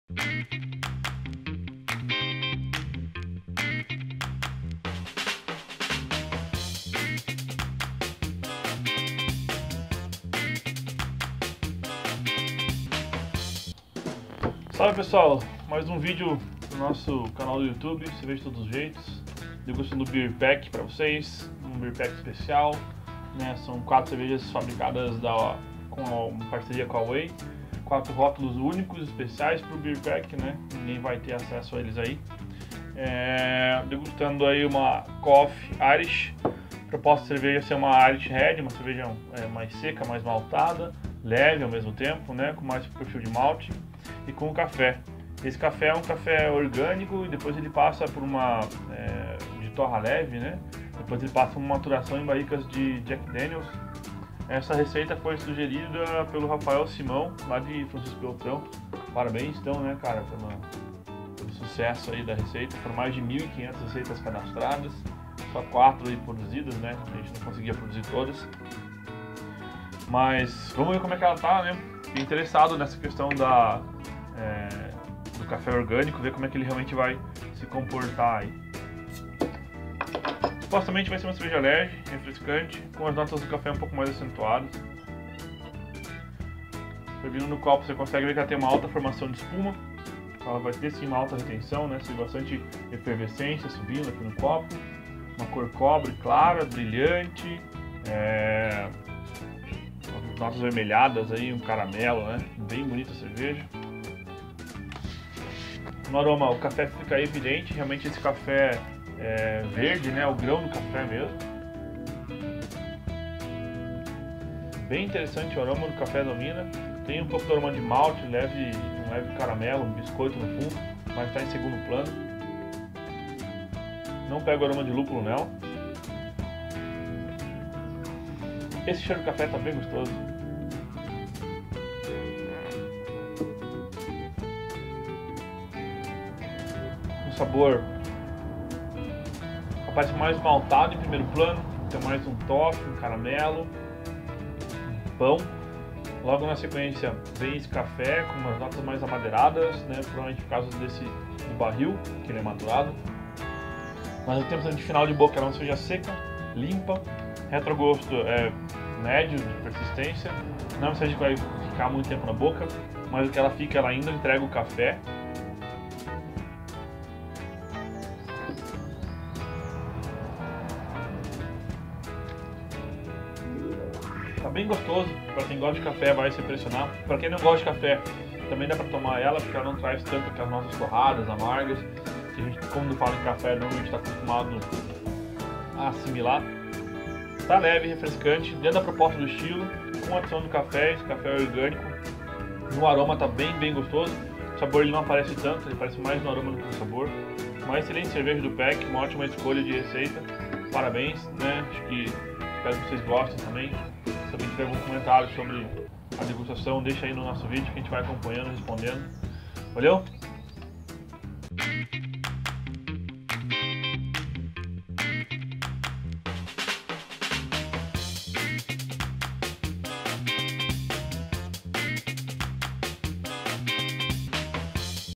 Way Beer Coffee Irish - Degustação Beer Pack Novembro 2016
No mês de novembro, de 2016, o Beer Pack traz 4 receitas da Way Beer, com produção baseada em sugestões de nossos clientes.

Neste vídeo, confira a degustação da Way Beer Coffee Irish, uma Irish Re d Ale com adição de café. Receita enviada pelo Rafael Simão, de Francisco Beltrão (PR), é uma cerveja mais seca, de coloração proveniente dos maltes especiais, com adição de um dos melhores cafés do Paraná.

Ainda não é assinante? Receba uma seleção de cervejas todos os meses na porta da sua casa